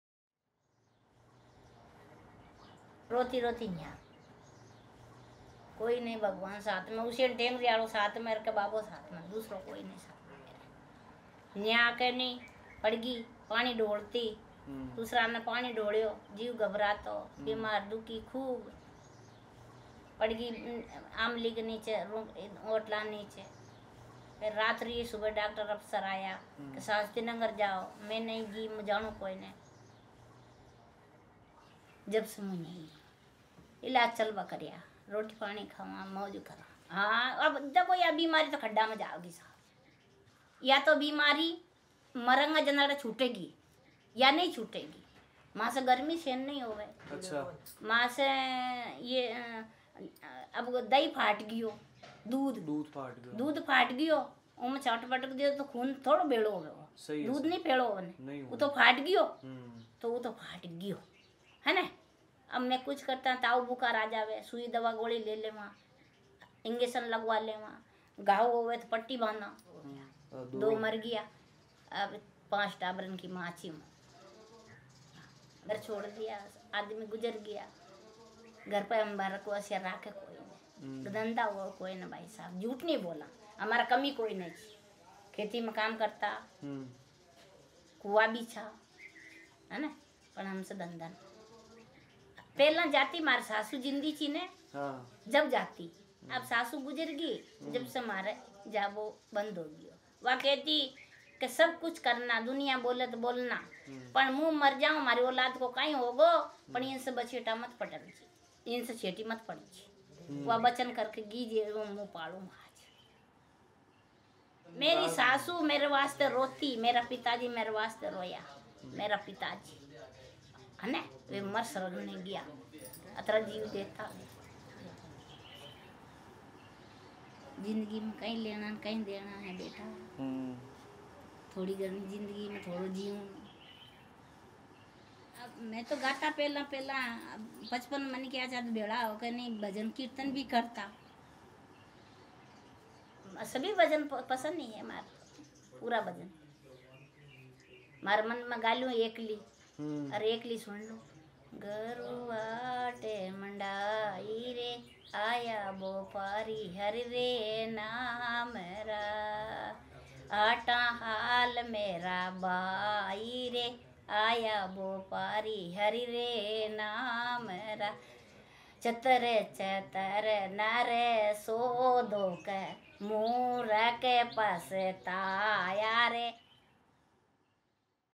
BASANTI DEVI INTERVIEW
Meet Basanti Devi and hear her story. She has been at the ashram for 43 years and finds special calm in devotional songs.

Basanti Devi was photographed and interviewed for the documentary short film SHUDDHI at the Nimba Nimbadi  hospital for Hansen's Disease in Jodhpur, India.

We chose to use still photos instead of cutting the video for the interviews. I felt the stills more strongly emphasized the expressions and feelings of the stories.